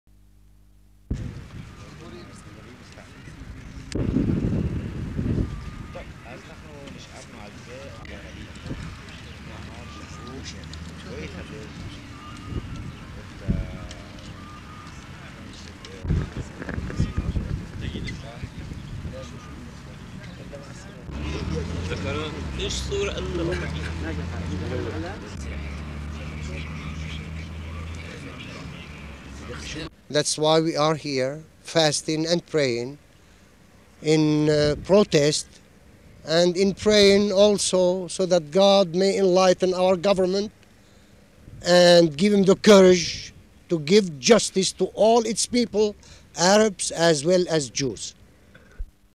SYND 17-7-73 ARCHBISHOP RAYA FASTING OUTSIDE THE KNESSET
(17 Jul 1973) The Melkite Greek Catholic archbishop Joseph Raya begins his fast outside the Knesset, Israel's parliament, to highlight his desire for Middle East peace.